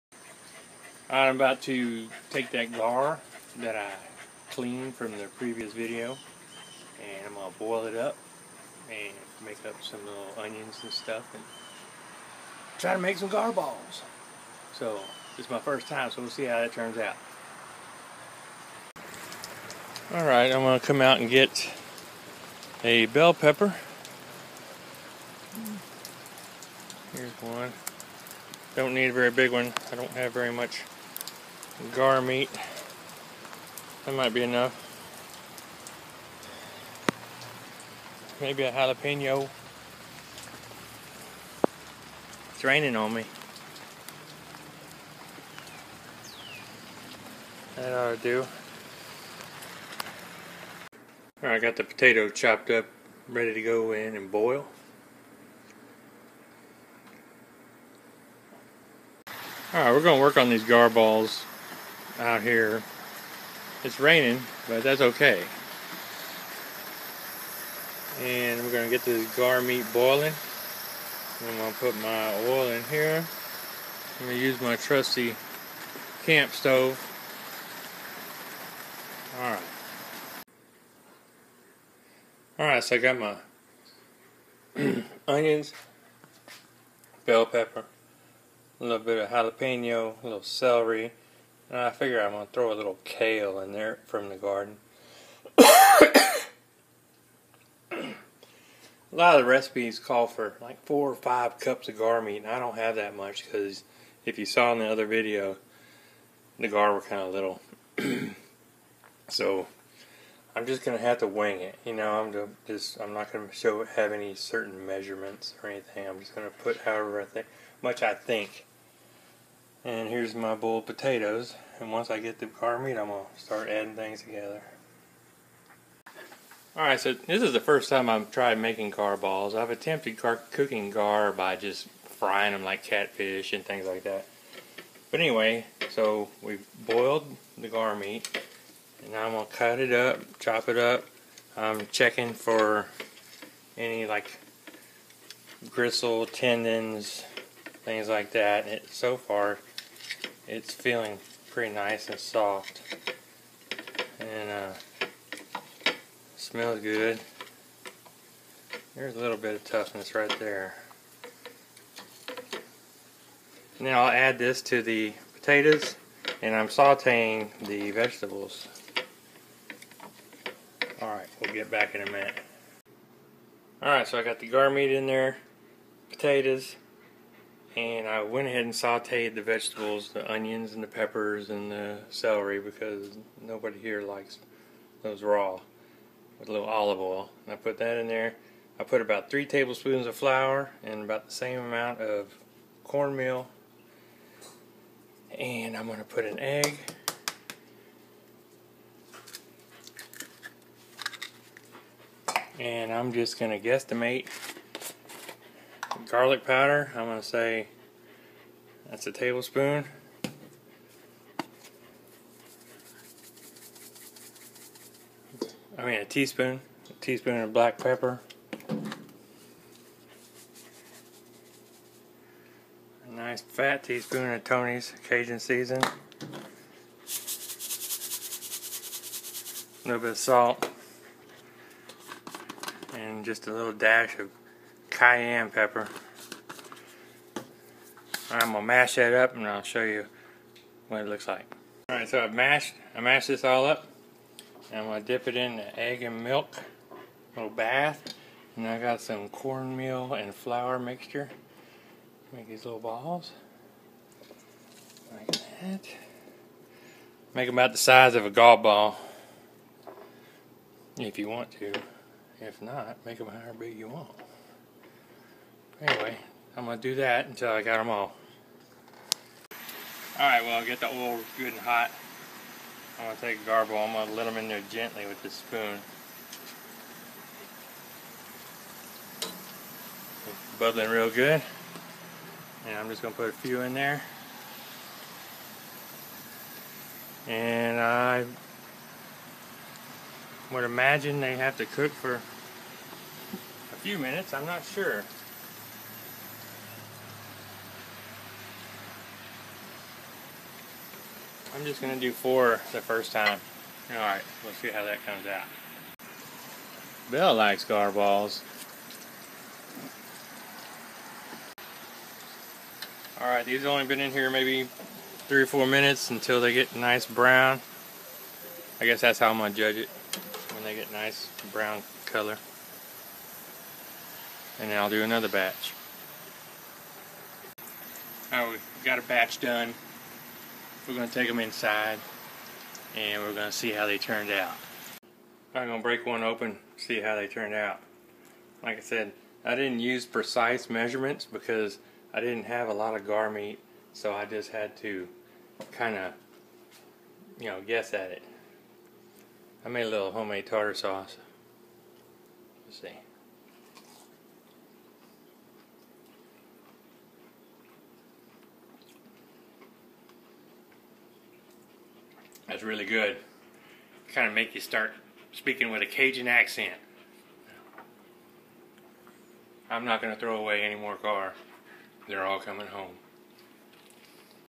Preparing GAR BALLS ( Boulettes), From Alligator Gar
I am making Gar Balls for the first time. I caught the gar in my previous video. They turned out awesome.
You can get all this on Amazon pantry

The simple tarter sauce:
three Tbls. Mayo,
 one Tbl. sweet pickle relish,
 1/2 tsp. lemon juice.
 black pepper,
cajun season,
garlic